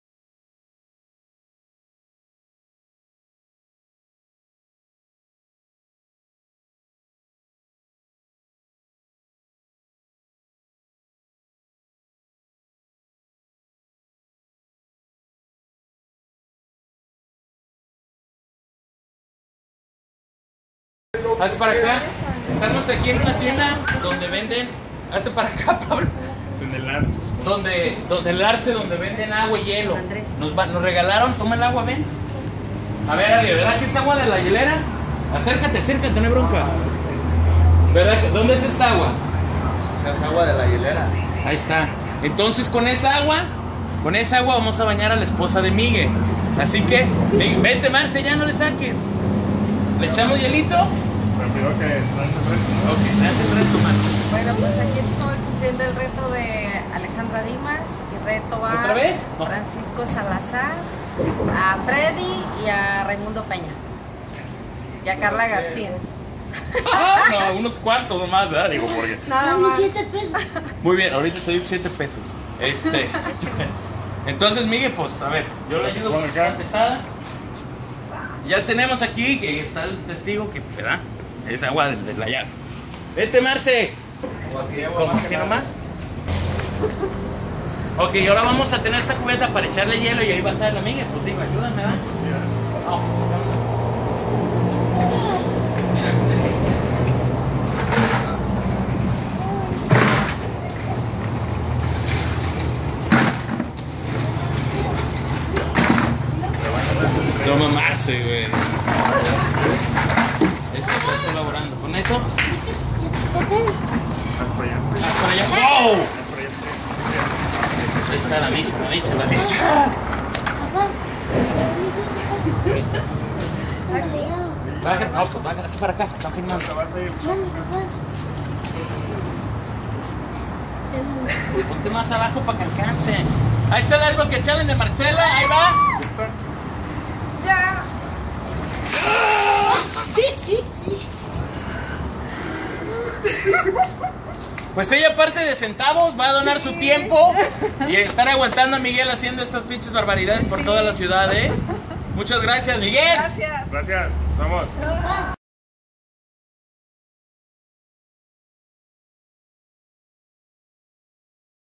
RetoCubetazoPorMiguel Marcela Olivo
Miguel tiene un hemangioma en la mejilla derecha, (irrigación excesiva de sangre), el cual está creciendo sin control, por lo cual requiere una cirugía muy costosa que asciende a $36, 500 dólares (alrededor de $500 mil pesos) por tal motivo lanzamos el RetoCubetazoPorMiguel para apoyarlo a costear la operación.
Súmate!!